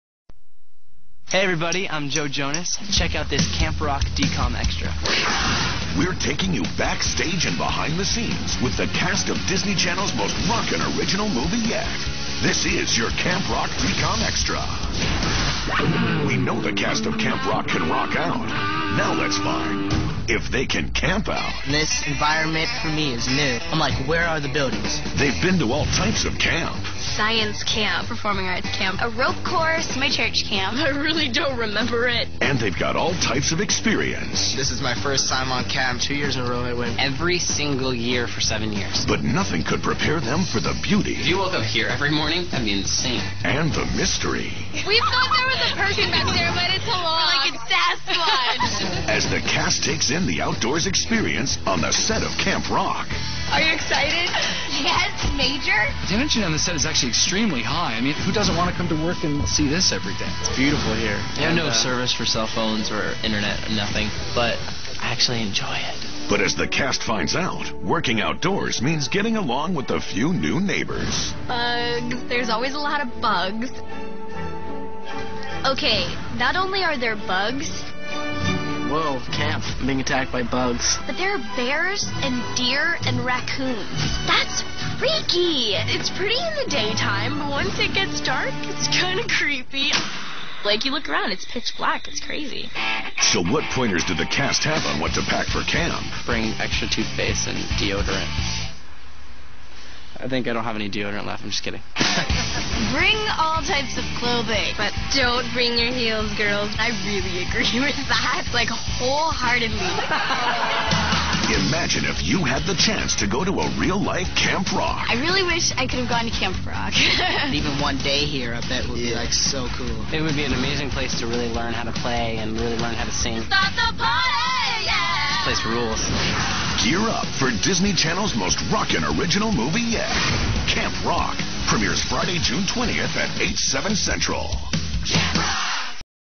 Backstage Camp Rock
Hey everyone this is Alyson Stoner and this video is also from when we were filming Camp Rock hope you like it,

~ Alyson ~